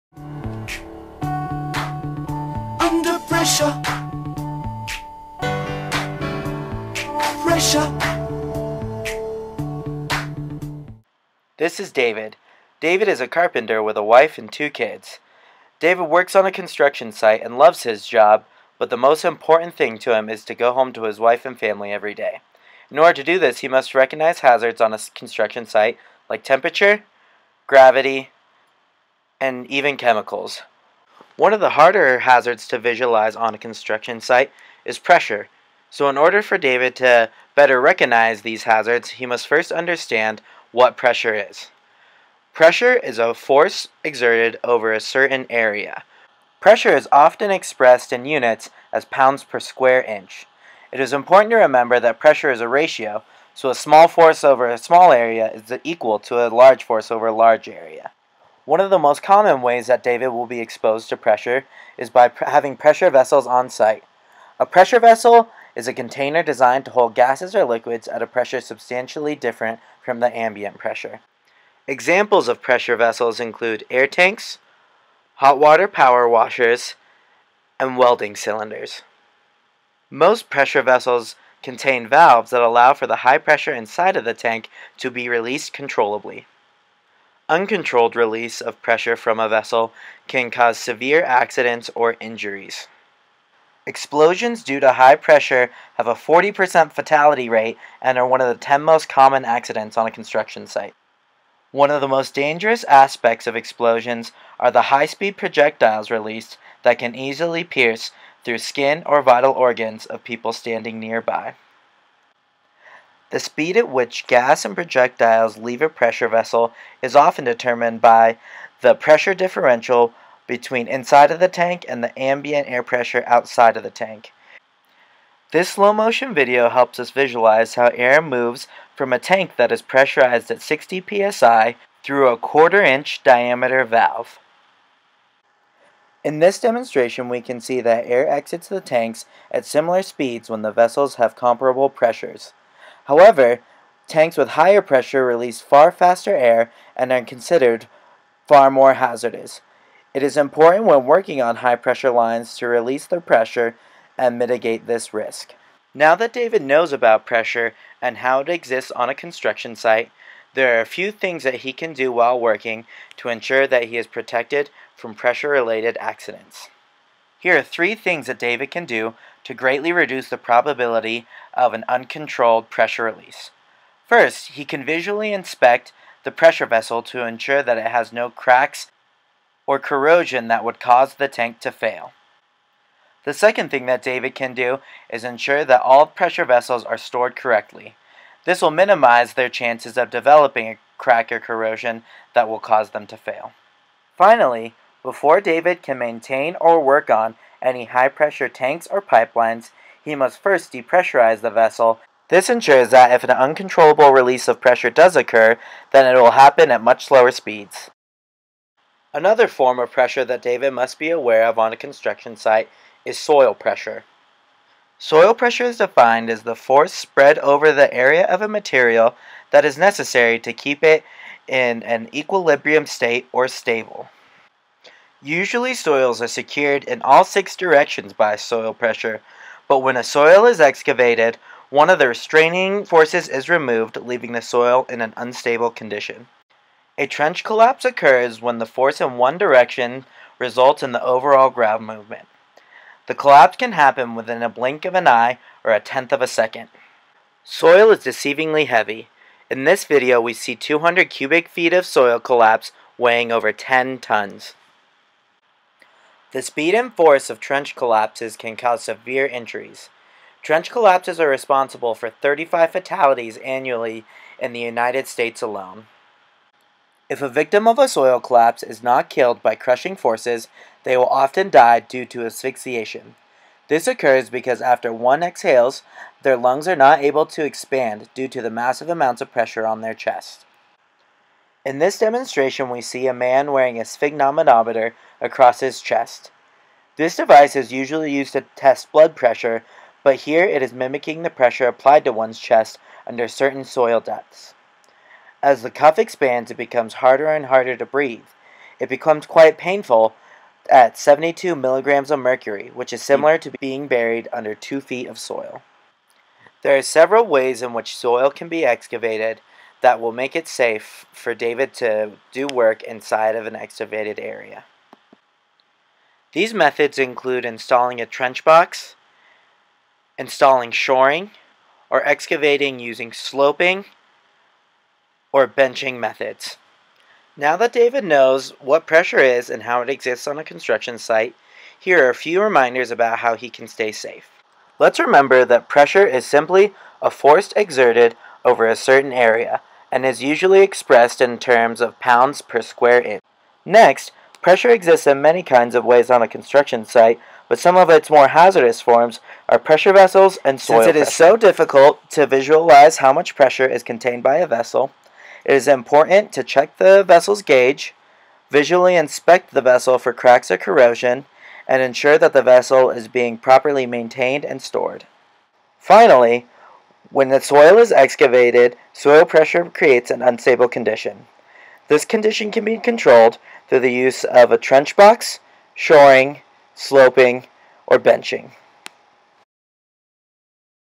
Pressure Hazards
Live Safety Demo for Pressure Hazards. Credit: Shawn Sweeney, Justine Manzec, and Matt Sears